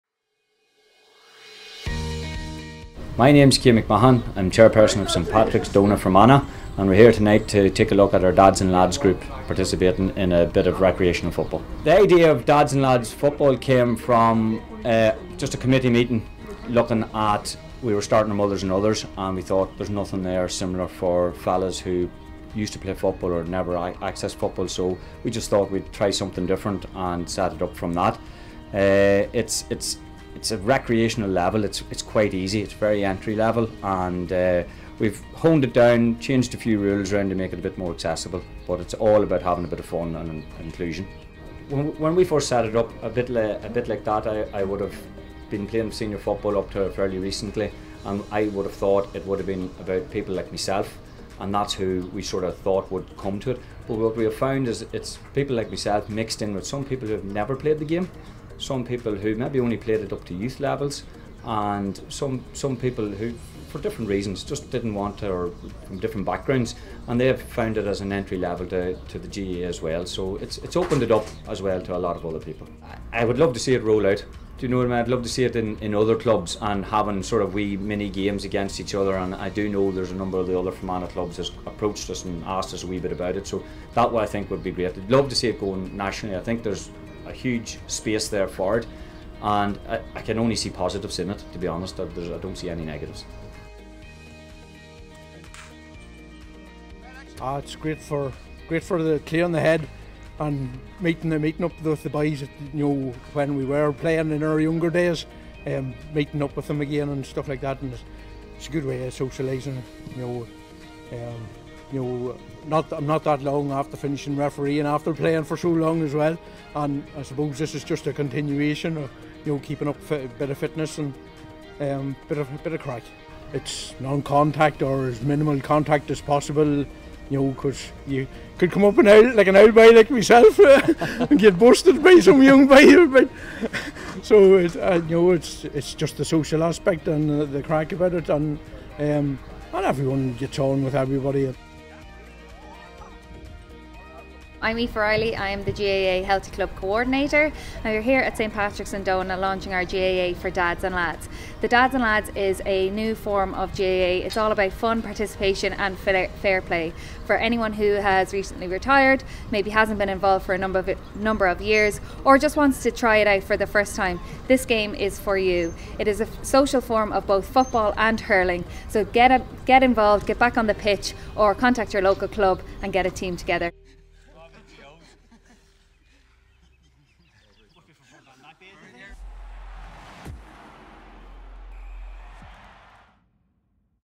'GAA for Dads & Lads' initiative launched.
‘GAA for Dads & Lads’ is a social hurling and football programme for those keen to enjoy Gaelic Games outside the traditional competitive structures of the GAA.